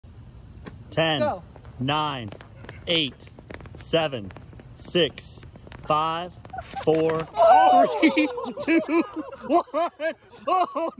Light-Painting Spill || ViralHog
Occurred on December 31, 2022 / Louisville, Kentucky, USA

"We were light painting, a long-exposure method used to create streaks of light behind a couple.  We asked two groomsmen, who had not been drinking, to come along and help and the third asked to be a part of it too.  When we did the countdown this time, the drunk one slipped and fell."